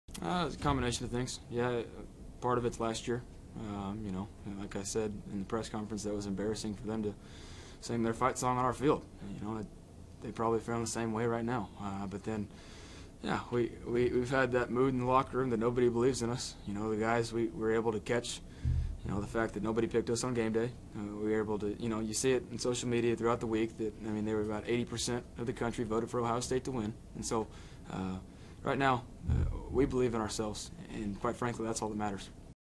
Baker Mayfield on pinning the flag: "Nobody picked us on GameDay" | ESPN
Oklahoma QB Baker Mayfield shares why he planted the Sooners flag on the Buckeyes' midfield after defeating Ohio State.